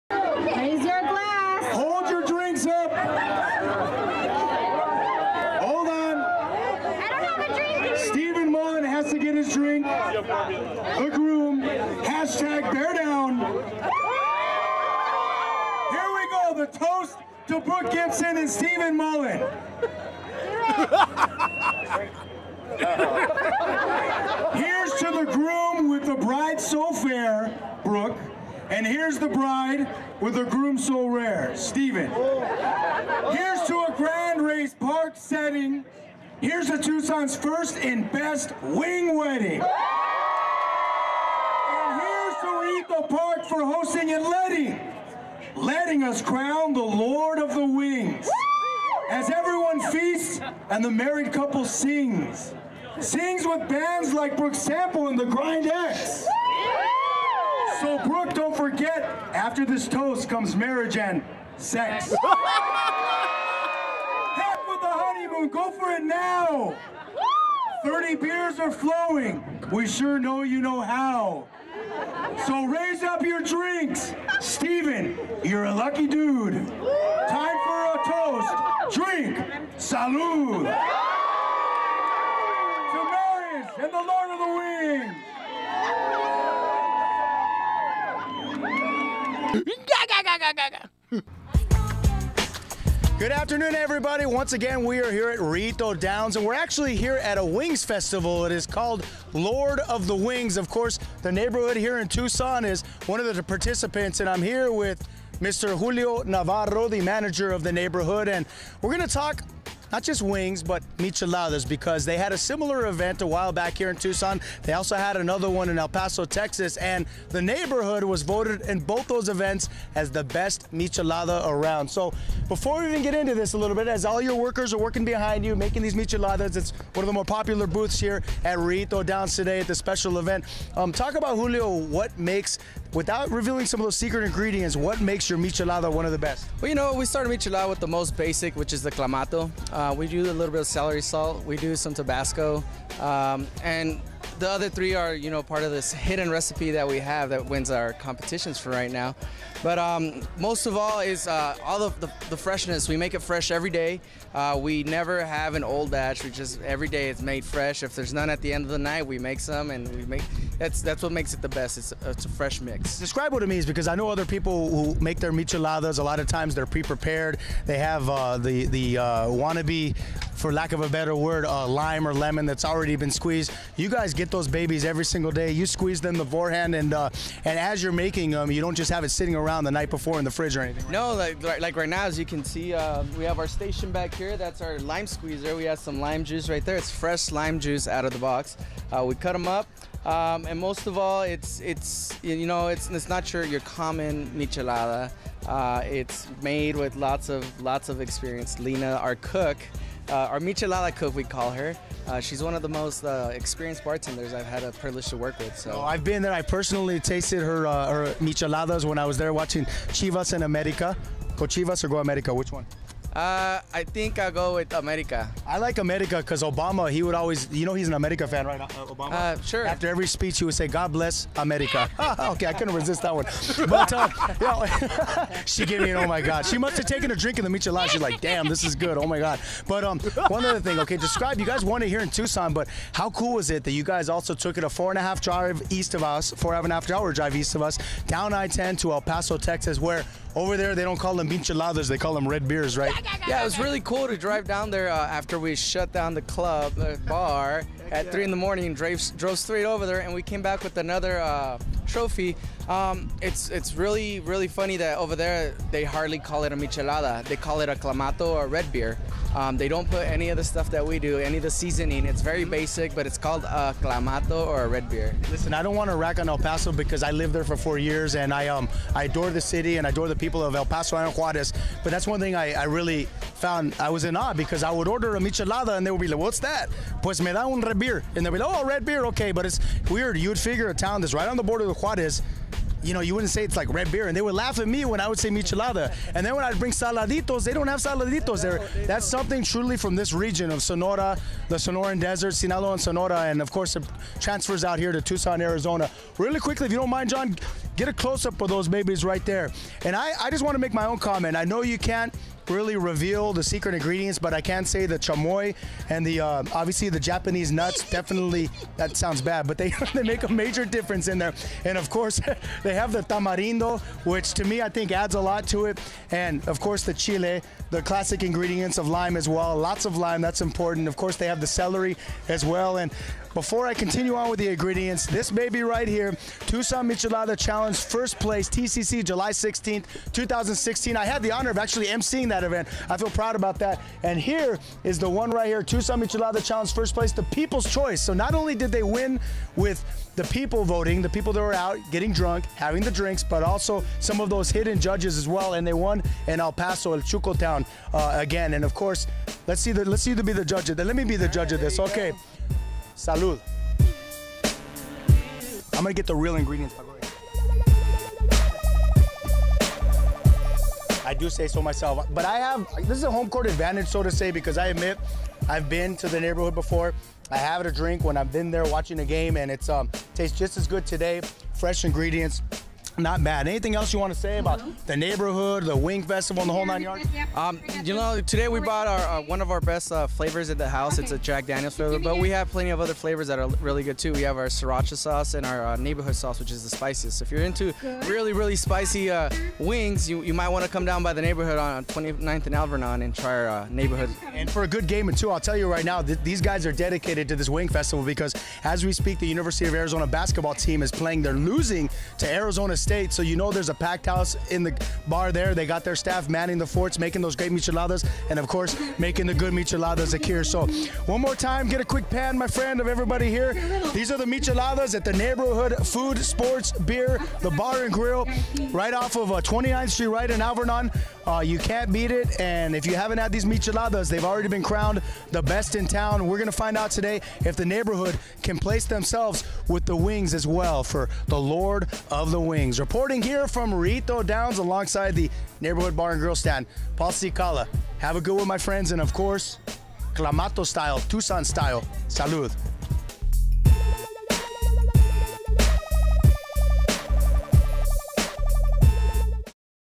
Paul Cicala giving "Toast" for "Wing Wedding Marriage" & Michelada Challenge
The Rillito Park Racetrack saw a very special and unique wedding ceremony on March 4th, 2017 as Steven & Brooke Mullen had a "Wing Wedding"!.  Paul Cicala gave a creative "Toast" for the Married Couple minutes after tying the not in front of hundreds of people.  Also, Paul Cicala talks with the 3-time Michelada champions "The Neighborhood" on what makes great wings -and- great Micheladas at the "Lord of the Wings" event.  Proceeds went to restoration of Rillito Park In Tucson. :)